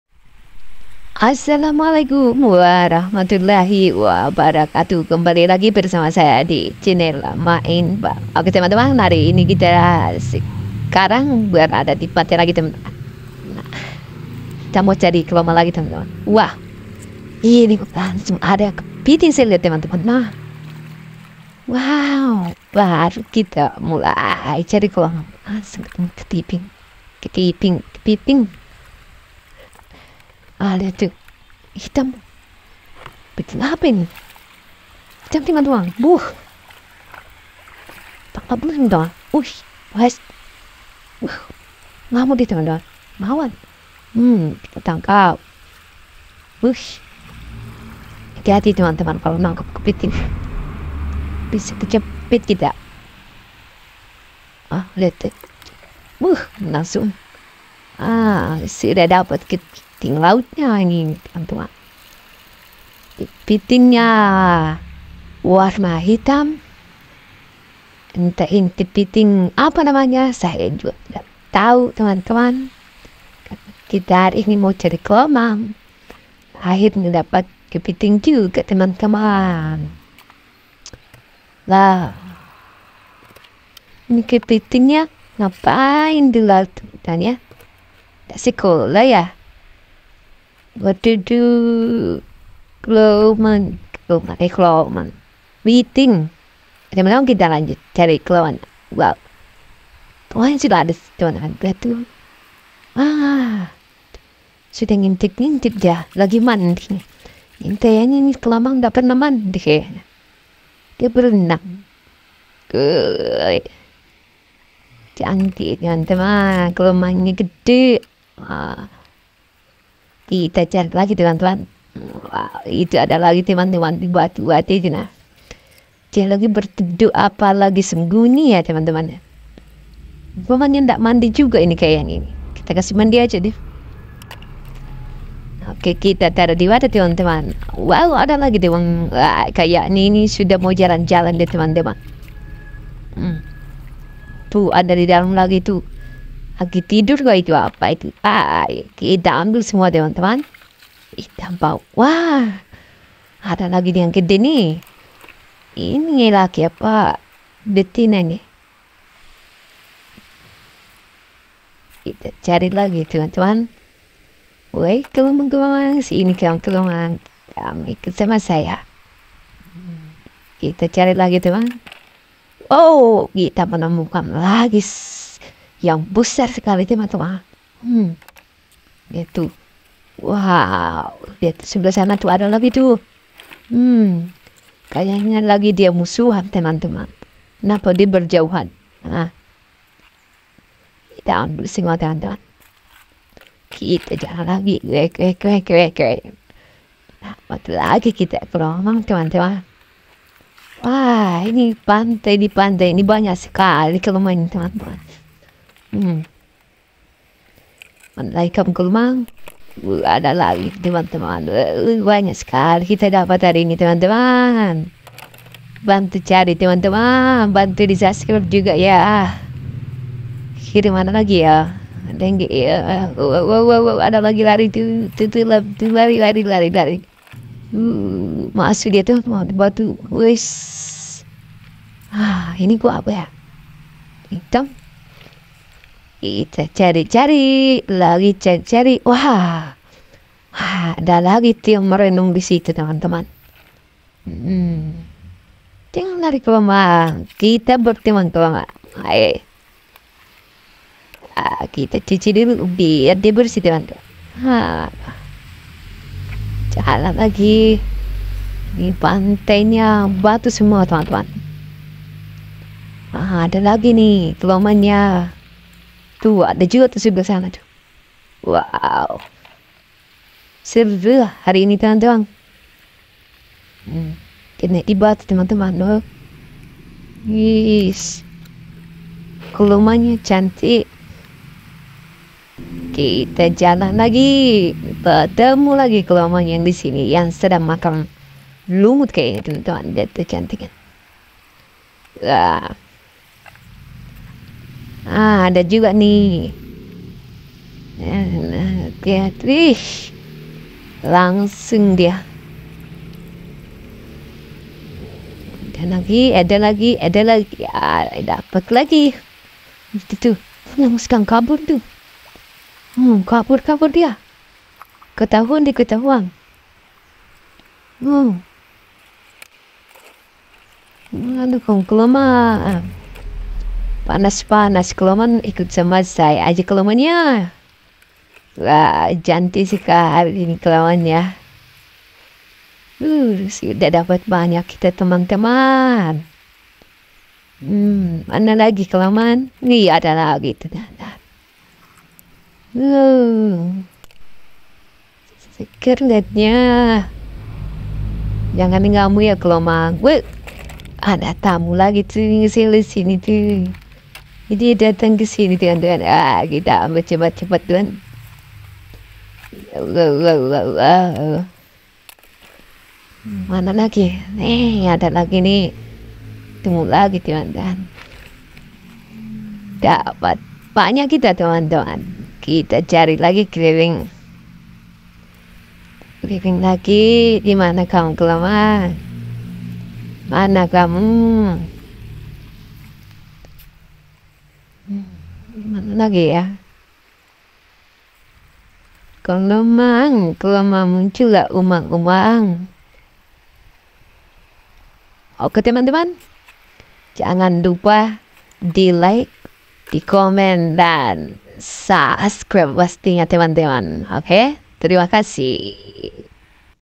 AYO MENCARI KELOMANG JUMBO DI PINGGIR PANTAI PANTAI
hello teman-teman hari ini saya mencari dan menemukan kelomang atau Umang-Umang di pinggir pantai yang indah
kelomang di sini Besar-besar/Jumbo dan lucu